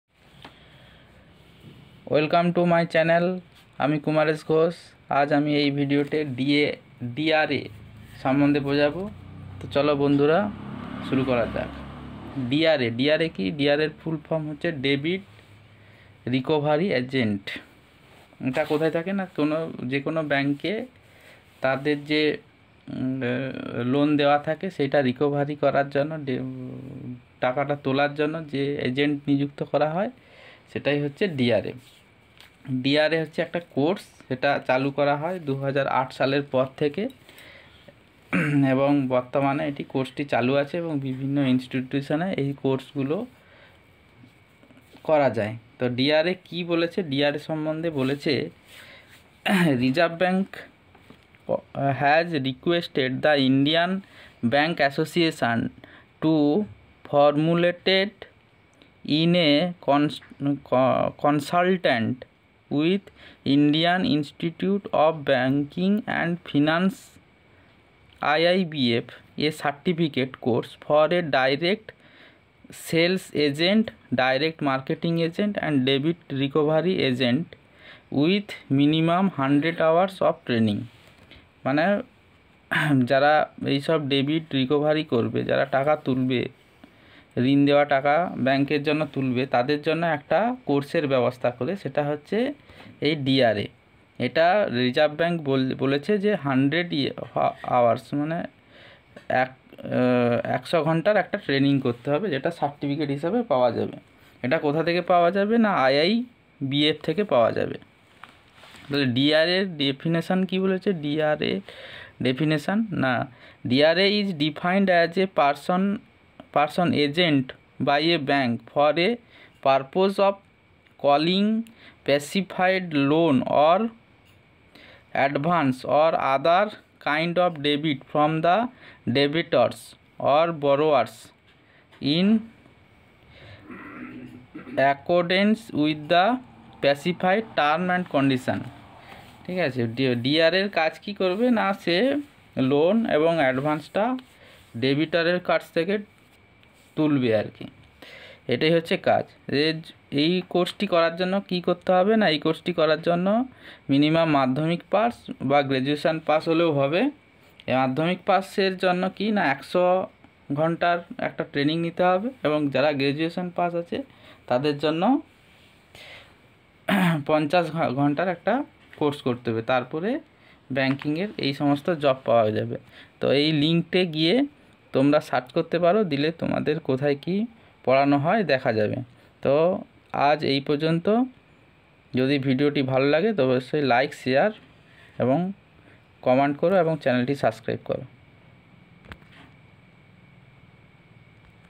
Debt Recovery Agent (D.R.A)
In this video I explained about D.R.A,.Dra means Debt Recovery Agent.